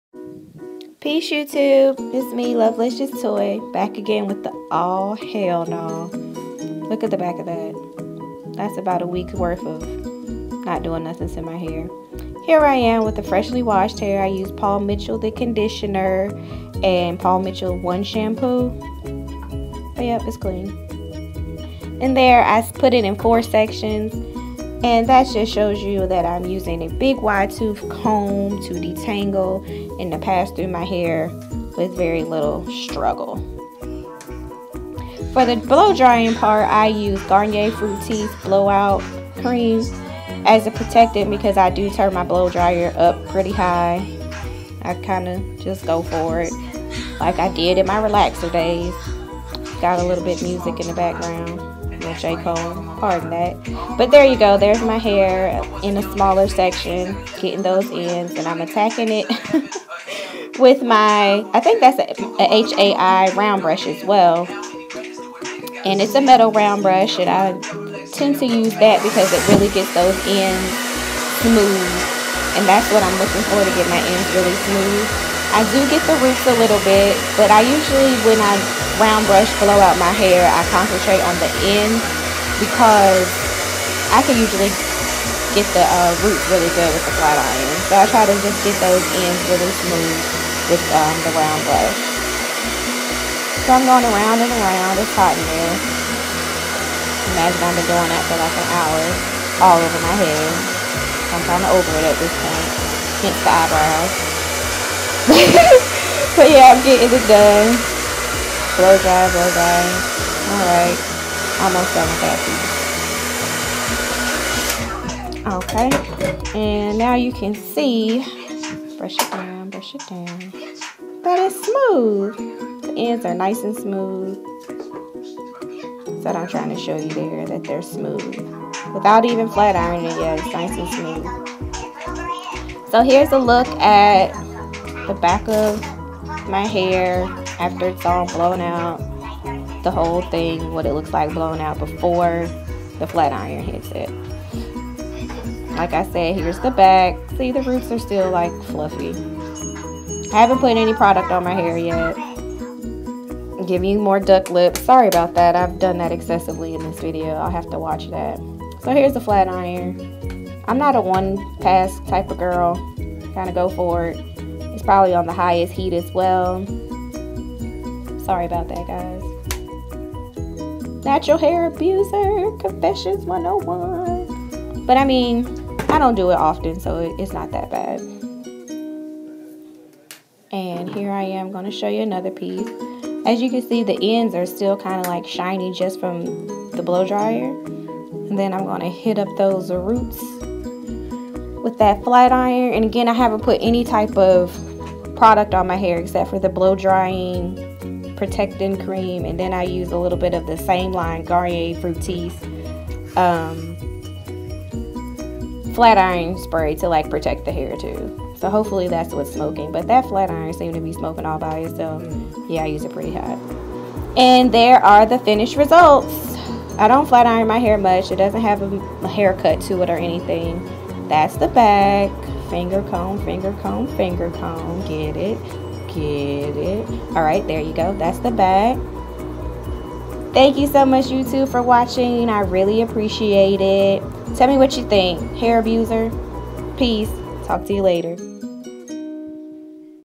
Round Brush Blow-Out (Curly Hair Abuse?)
Hey Guys Thanks for stoping by.  Here is a quick look at how I blow-out my hair.  This is not a tutorial...I am not an expert.  I am actually a very lazy naturally curly girl who loves youtube tutorials because I am pretty clueless when it comes to hair.  I never heard of heat damage (the kind that changes curl patterns) until I started watching YT.  But it didn't stop me from "busting" those curls with high heat.  I know I know shame on it all...lol... Hope you like the Video!